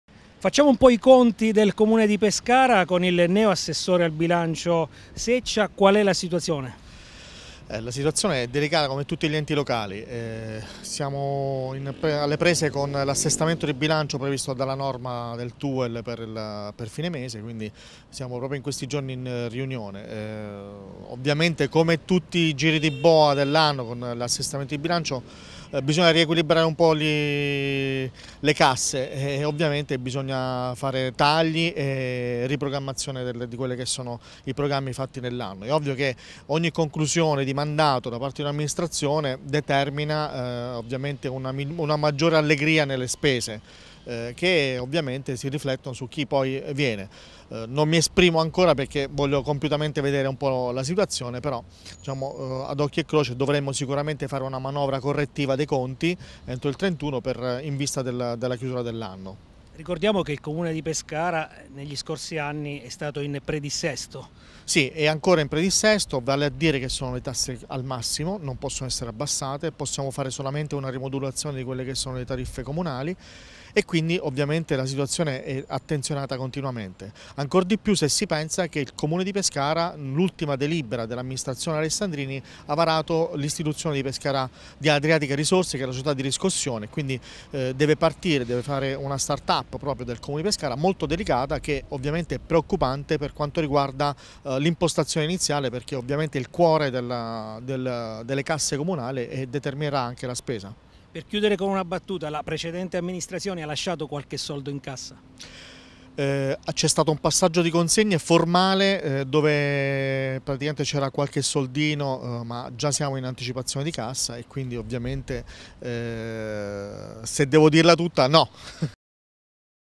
Pescara L'assessore Seccia fa i conti al Comune
Pochi soldi in cassa per il Comune di Pescara e tagli in vista. Lo spiega in questa intervista il neo assessore al Bilancio Eugenio Seccia.